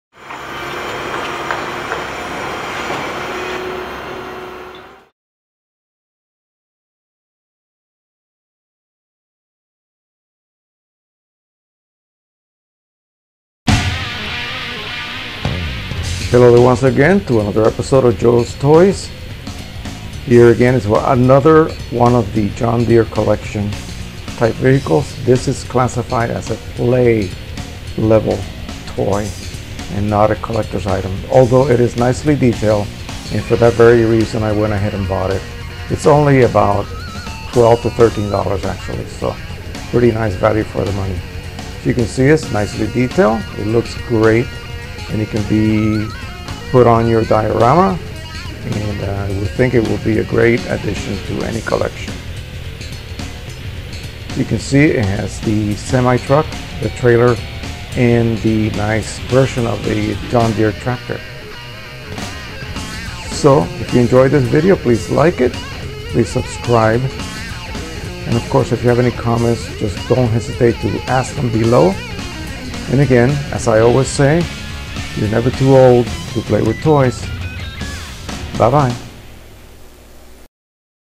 My New Love - Die-cast toy 1:50 scale Model of a John Deer Semi and Flat Trailer and Tractor
Yet another one of the models I bought at the John Deer tent at my local county fair.
This one is a generic "John Deer Semi truck, trailer and tractor being used as a Trailer Cargo Load